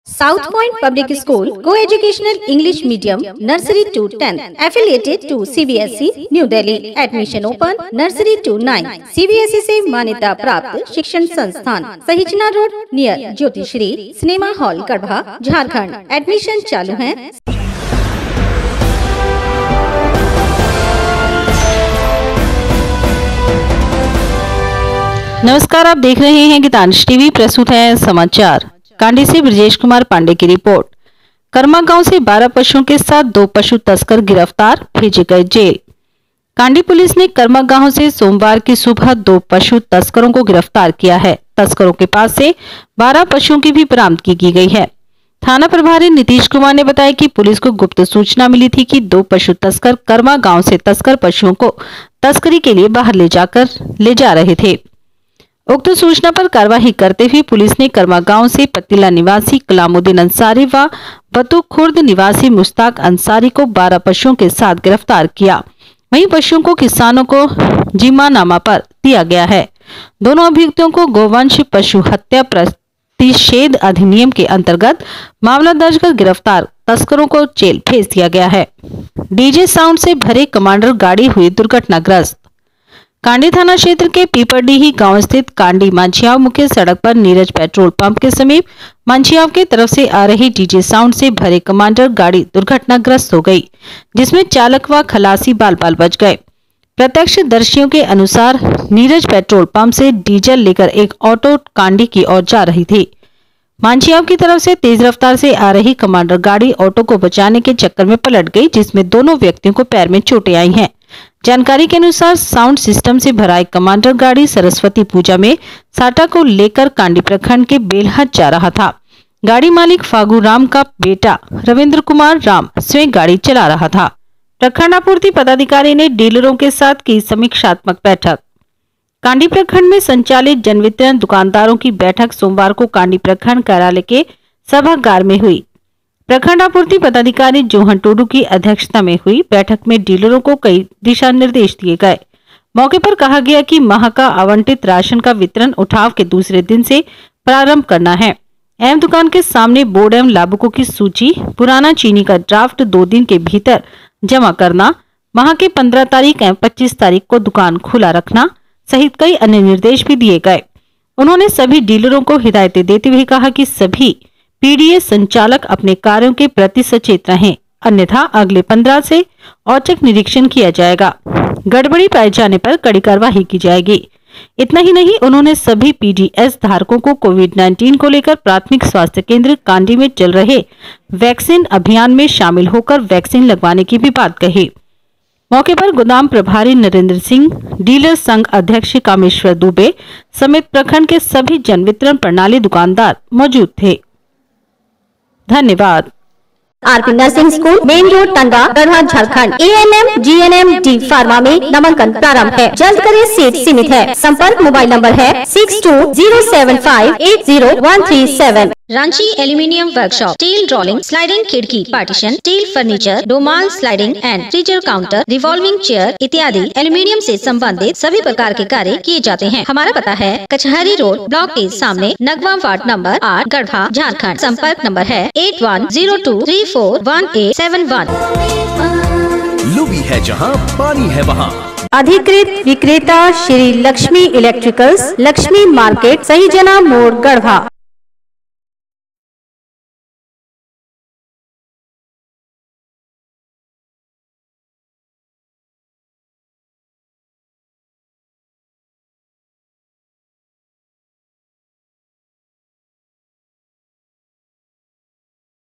Kandi News 15 02 2021
1-कर्मा गांव से 12पशुओं के साथ दो पशु तस्कर गिरफ्तार, भेजे गए जेल
2-डीजे साउंड से भरे कमांडर गाड़ी हुई दुर्घटनाग्रस्त
3-प्रखंड आपूर्ति पदाधिकारी ने डीलरों के साथ किया समीक्षात्मक बैठक